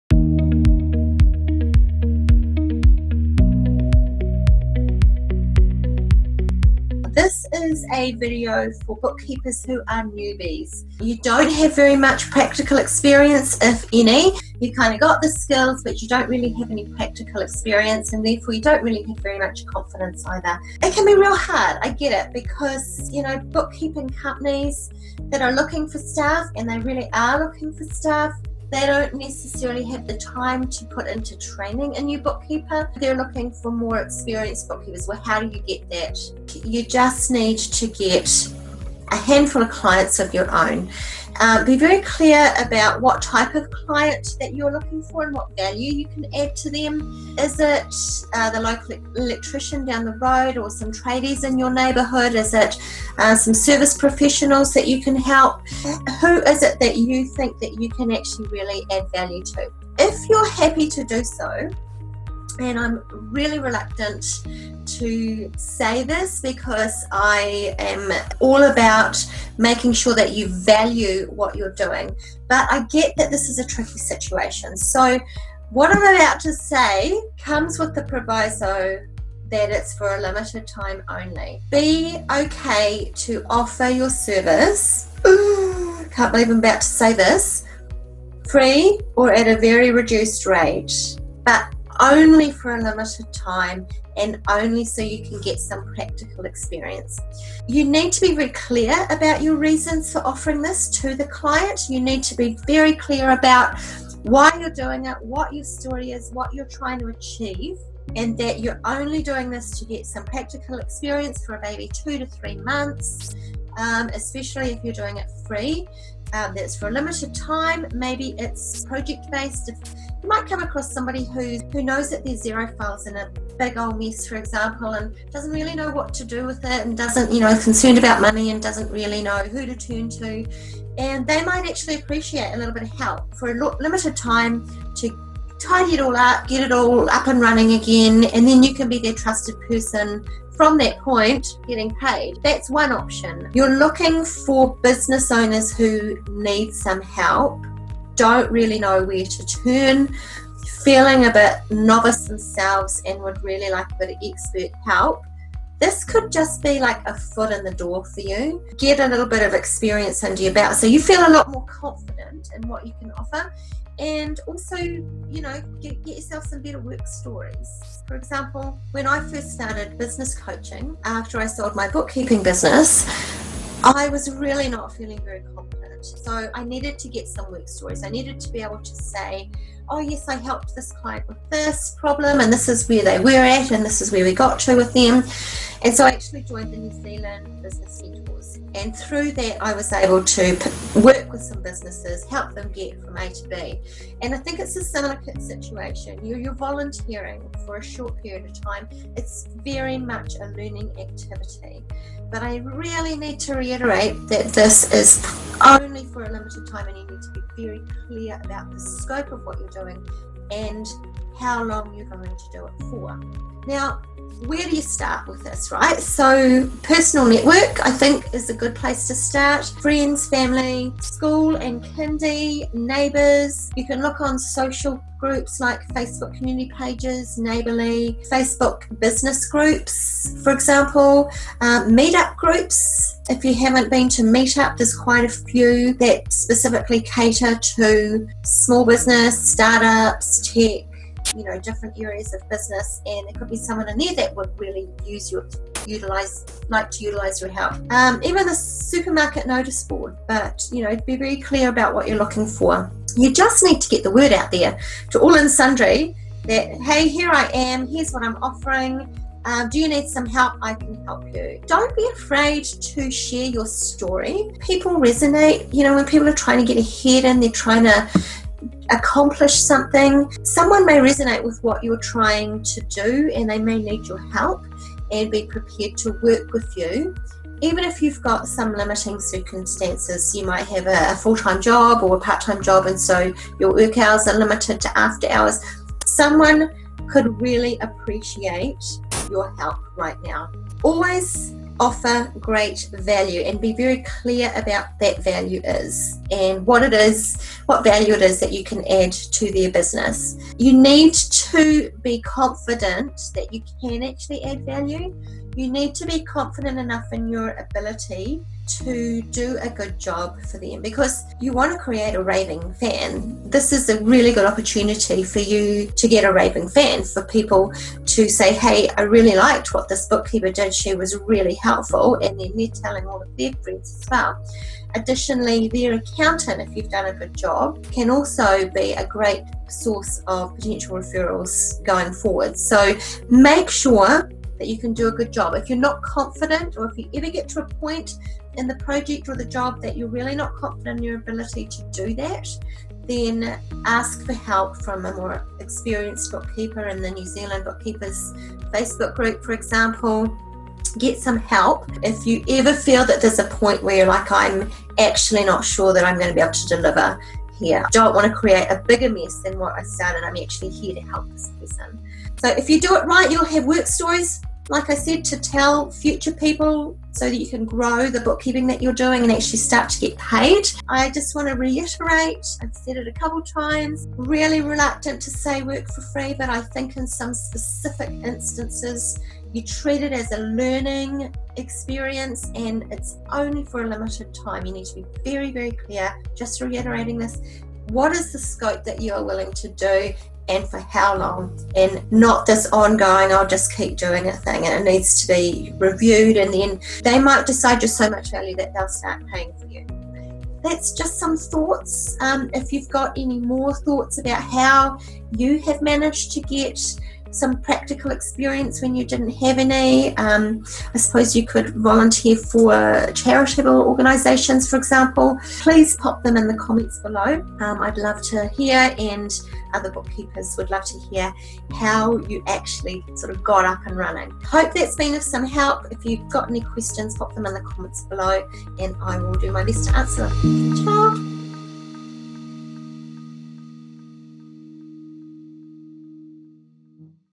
The catch 22 for new bookkeepers
For new bookkeepers it can be hard to get some practical accounts experience. It can be a bit of a catch 22, right? You can’t get a job without practical experience and you can’t get practical experience without a job.